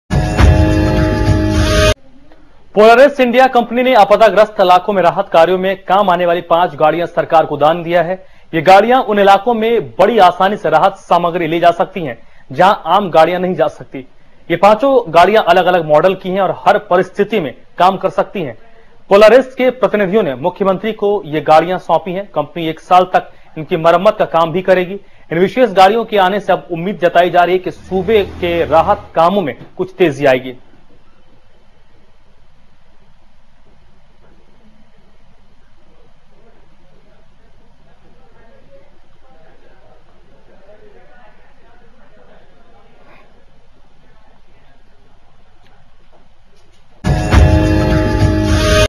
Polaris India donates five vehicles to Uttarakhand government
Polaris India Pvt Ltd, a wholly owned subsidiary of Polaris Industries Inc., the world leader in off-road and All-Terrain vehicles has donated five off road vehicles to Uttarakhand Government today, to assist in relief efforts after the state was hit by floods recently. Polaris Industries Inc. US has also supported this cause.
Polaris has donated one Sportsman Big Boss 6X6 800, one Sportsman 550 EPS, one RZR S 800 EFI and two Ranger 900 Diesel. The vehicles were handed over to Hon'ble Chief Minister, Shri Vijay Bahuguna of the state at Uttarakhand secretariat, Dehradun. Adding to the gesture of commitment to stand by the government in this social cause, Polaris has also offered to maintain these vehicles for a period of two years with full service and repair at no cost to the government.
"We are moved by the magnitude of the recent tragedy in Uttarakhand. With the entire network of roads in and around the states destroyed and non-existent now, apart from debris on the ground from thousands of destroyed homes, we feel that Polaris India can extend a hand of support to help expedite the relief operations. These specialised Off Road Vehicles will help in making these areas accessible," said Mr Pankaj Dubey, MD, Polaris India.
Polaris vehicles are specially designed to manoeuvre around rough terrain where best of vehicles fail to run. The vehicles are widely used by defence, Para military forces & police, and disaster management, tourism and forest departments.
"Polaris has long supported charitable efforts, and we hope the donation of these vehicles will assist the relief efforts as they provide the affected communities with the relief they desperately need. We also offer to help train the rescue and relief teams about the usage of these vehicles and educate them about their capabilities," added Mr Dubey.